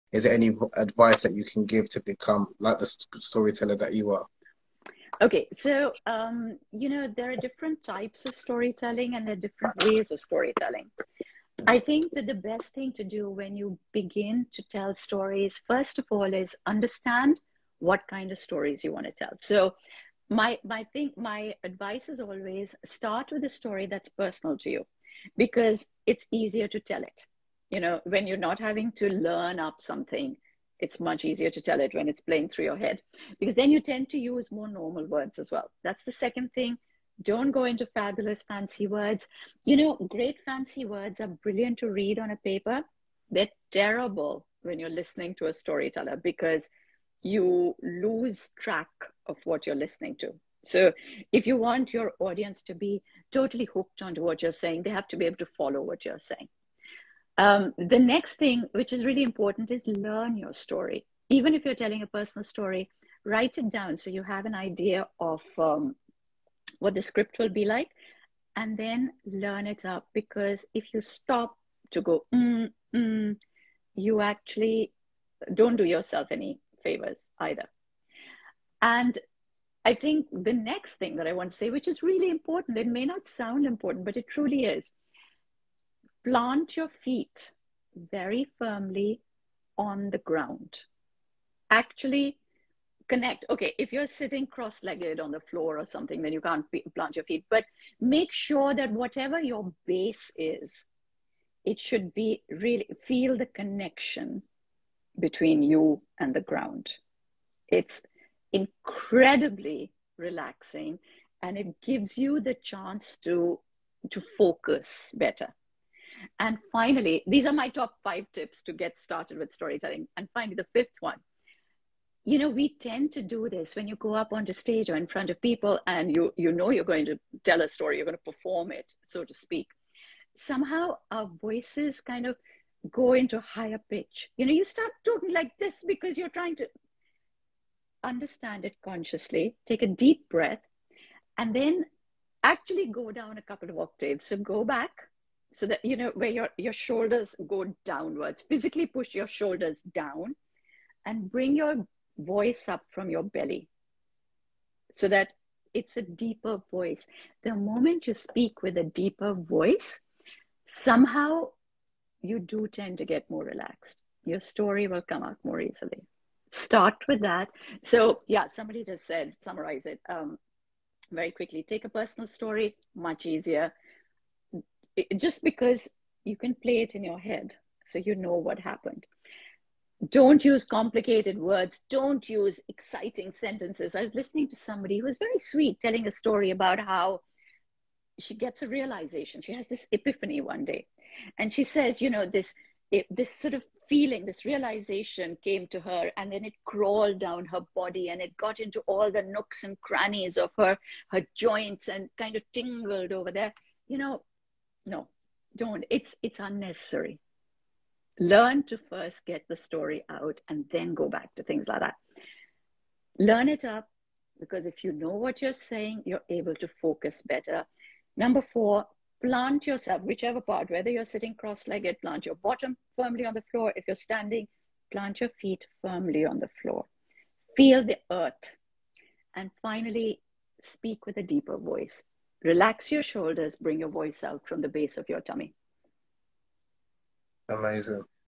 5 AMAZING Tips To Become A Master Storyteller
IG Live with Seema Anand, storyteller, mythologist and Kama Sutra expert.

Recorded on 30 June 2022 on IG Live of Habeeb Akande